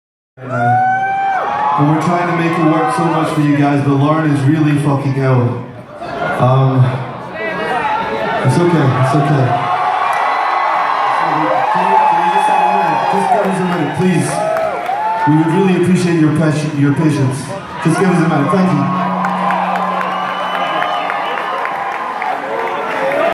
Chvrches Lauren is ill! 4/24/16
Band takes break mid song